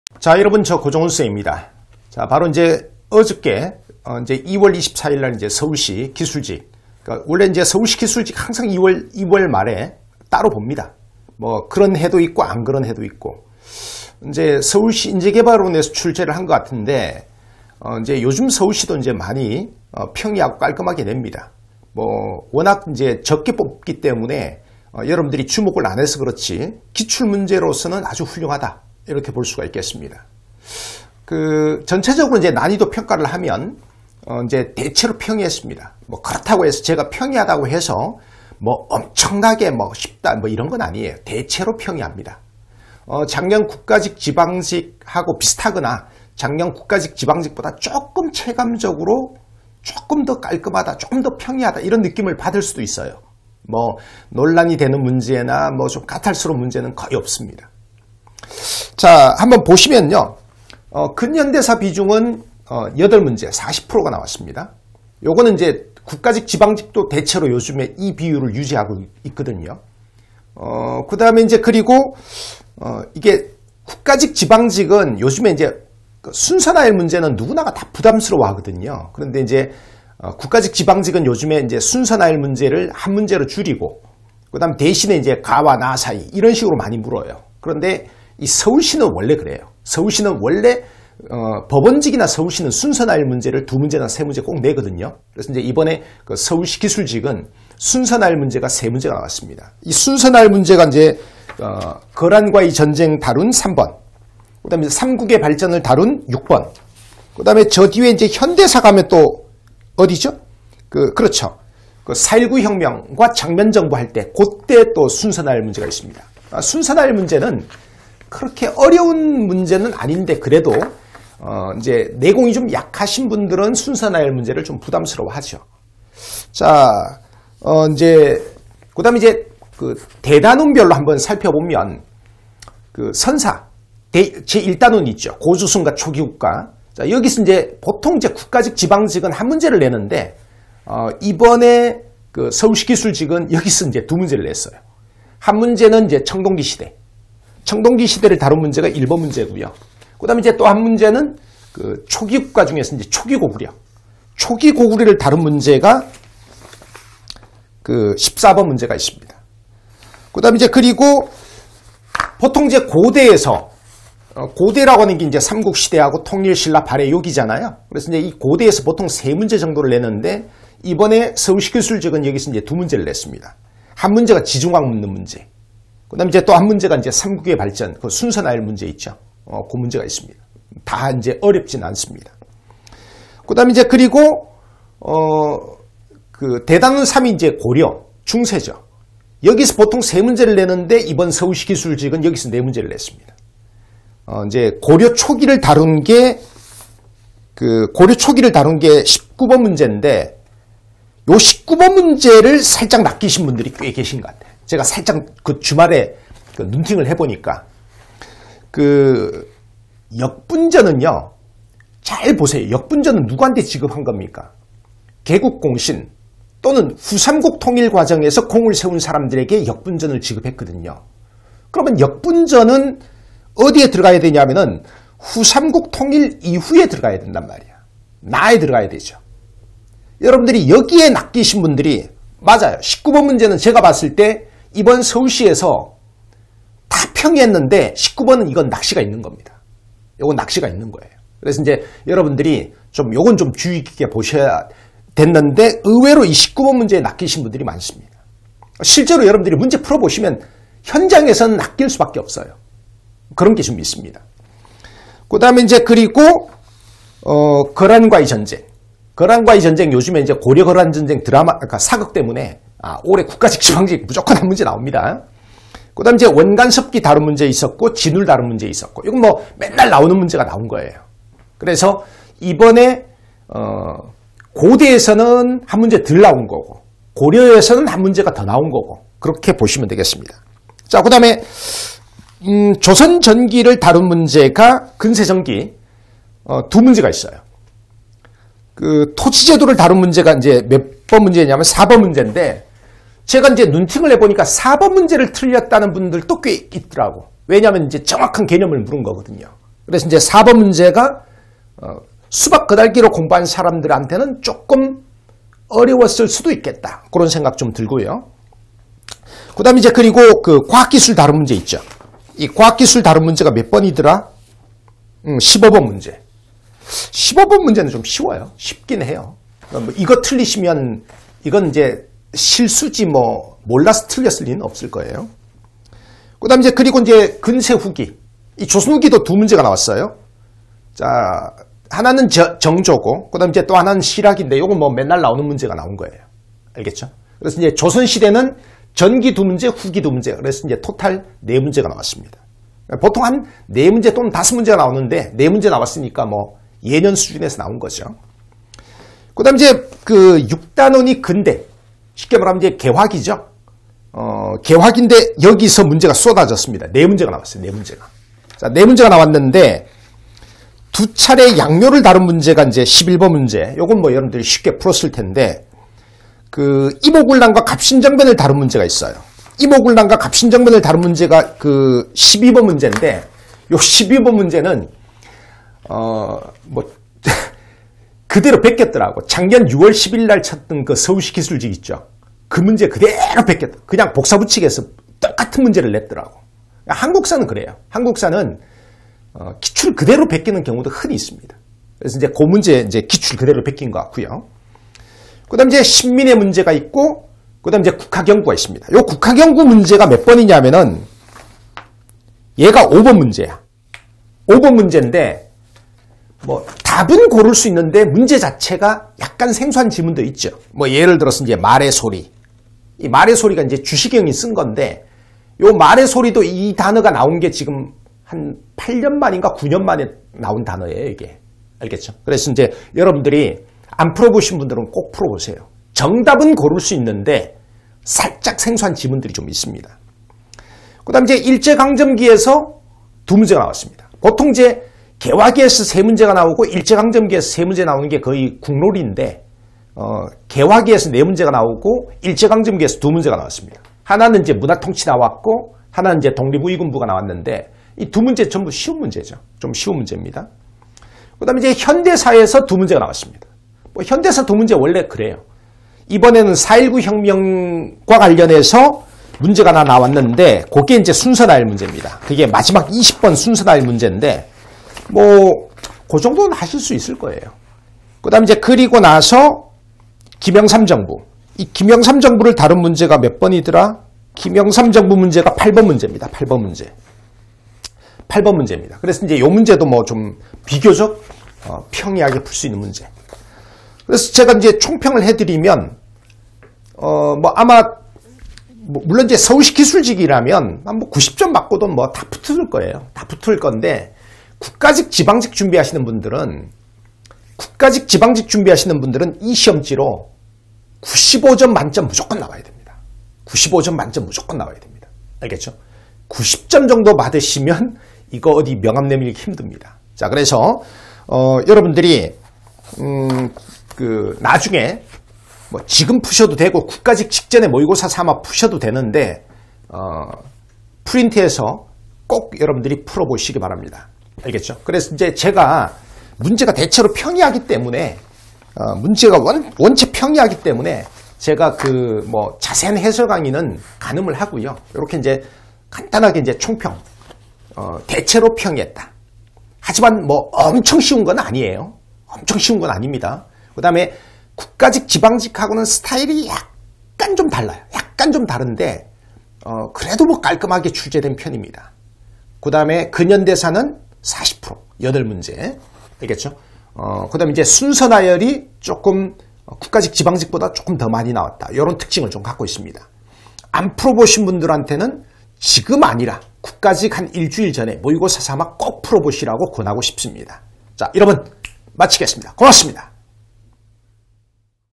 2024.02.24 공무원 한국사 서울시 기술직 총평
2024년 2월 24일 시행된 서울시 기술직 한국사 총평 입니다.